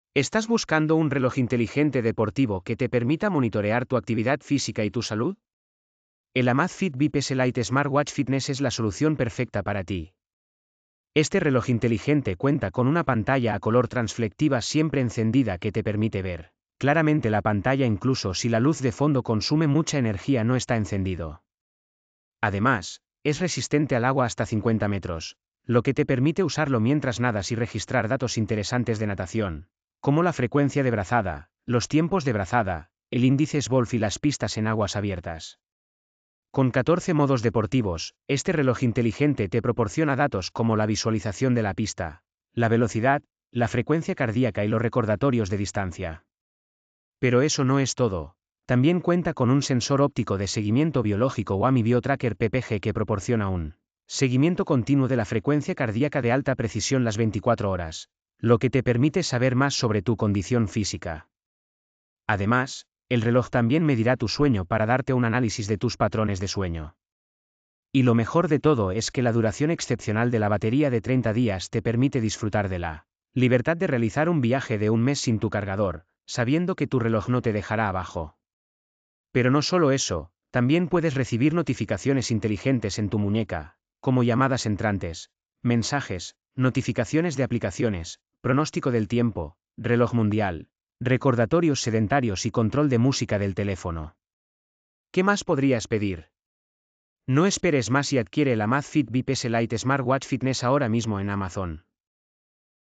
Reseña del Amazfit Bip S Lite Smartwatch - Duración de batería de 30 días, descubrelo en esta review
En esta reseña del Amazfit Bip S Lite Smartwatch, te mostramos todas las características de este reloj inteligente deportivo. Desde su pantalla transflectiva siempre encendida hasta su resistencia al agua de hasta 50 metros, este reloj es un gran compañero deportivo. Además, su duración excepcional de la batería de 30 días y su monitoreo de salud y seguimiento de actividad todo el día lo hacen perfecto para aquellos que buscan un reloj inteligente básico pero efectivo. ¡No te pierdas esta reseña!

Enlace del producto en Amazon